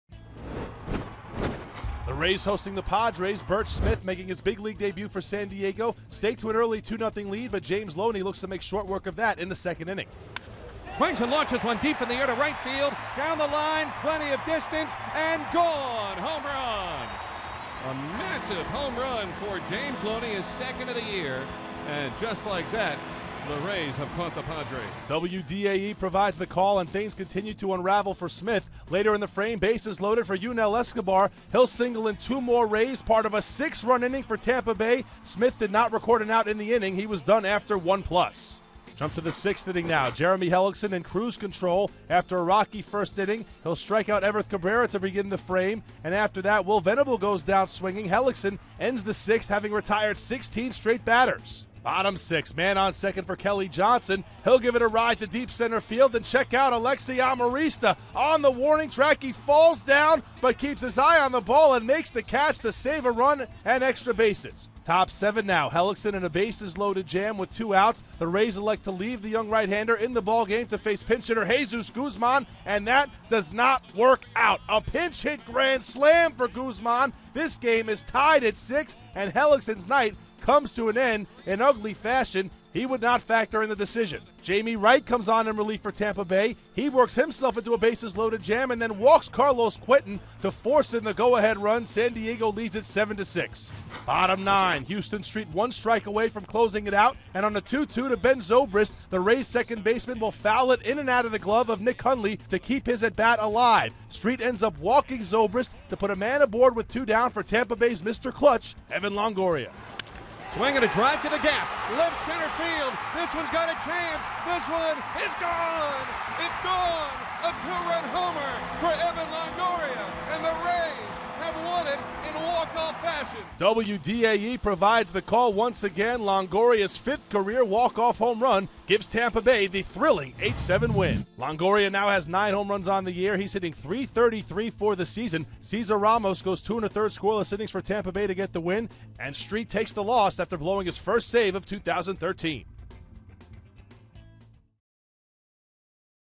2013/05/11 Recap: SD 7, TB 8
5/11/13: Padres battle back, but Longo walks off